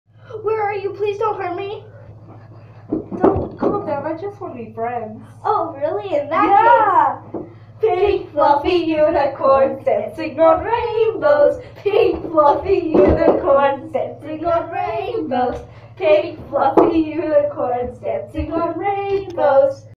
Bloody Mary challenge part 2
I figured out the Bloody Mary is real but she just wanted to be friends see what happens next in part three when we get 100 likes